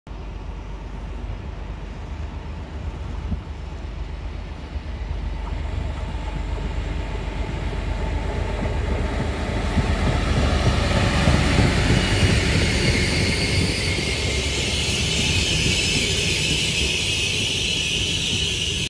IC 562 REGGIO CALABRIA-TARANTO
Arrivo dell'IC 562 Reggio Calabria-Taranto a Metaponto,con in testa la D445